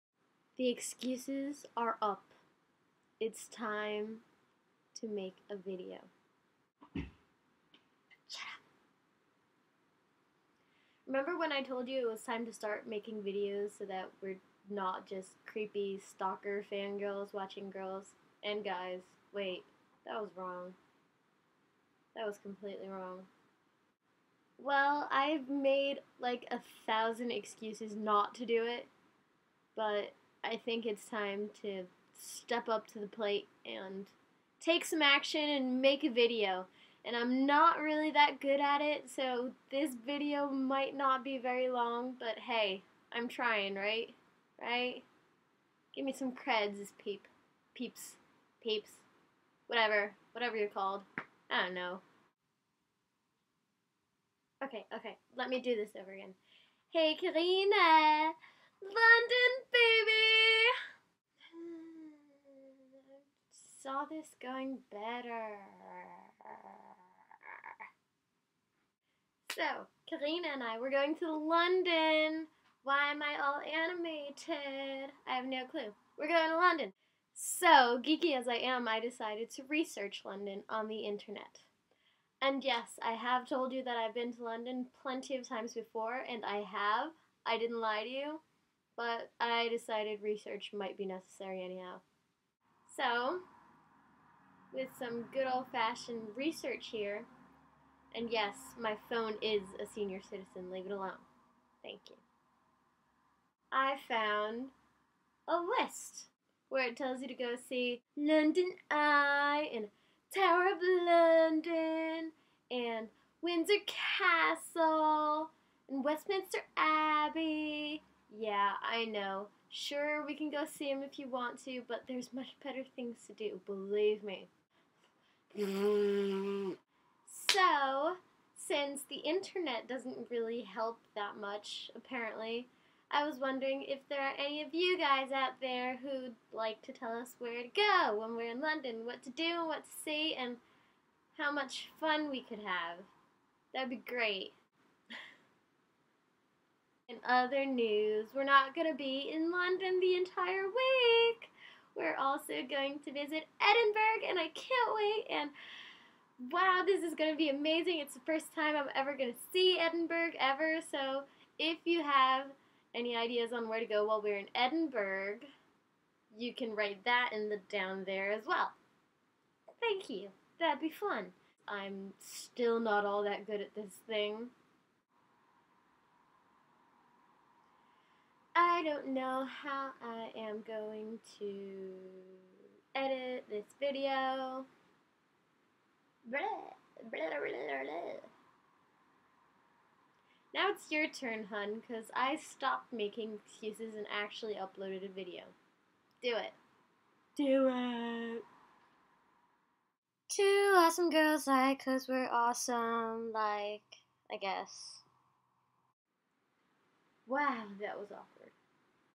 um... london baby?!
my macbook just happened!!! so i decided to film with it, and check out iMovie and other stuff.

wow. i really hope i get better at this sooner or later.